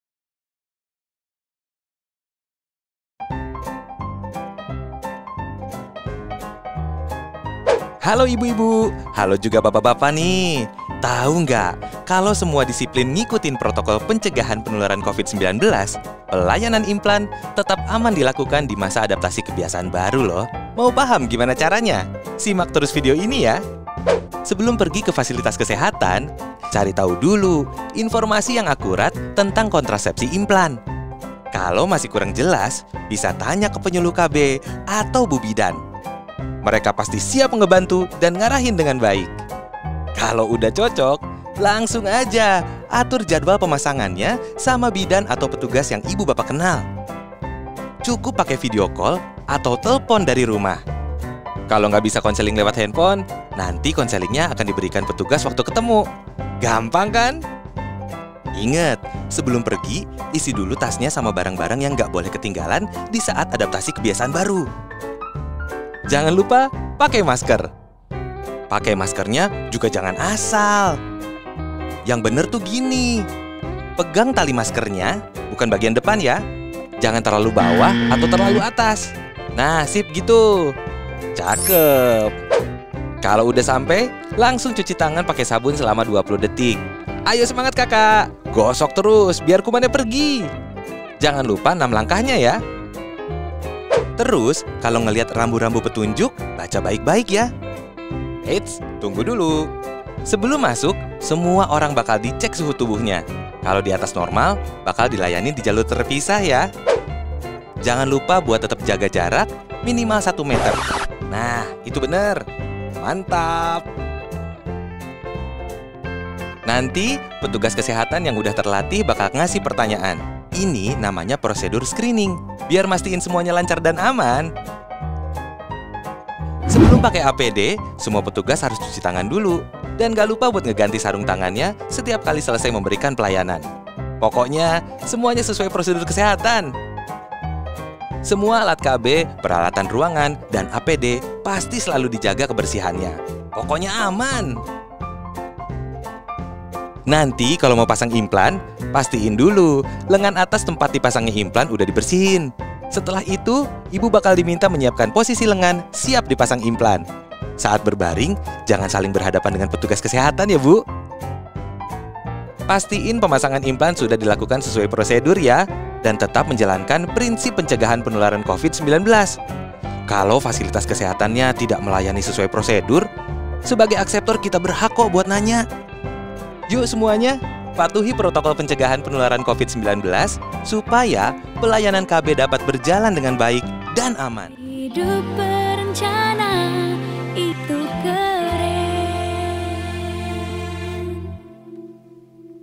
Cara Tetap Aman Akses Pelayanan Implan Saat Pandemi COVID-19
Video ini dibuat berdasarkan Buku "Panduan Pelayanan Keluarga Berencana dalam Masa Pandemi COVID-19 dan Adaptasi Kebiasaan Baru" yang diterbitkan oleh Kementerian Kesehatan dan BKKBN.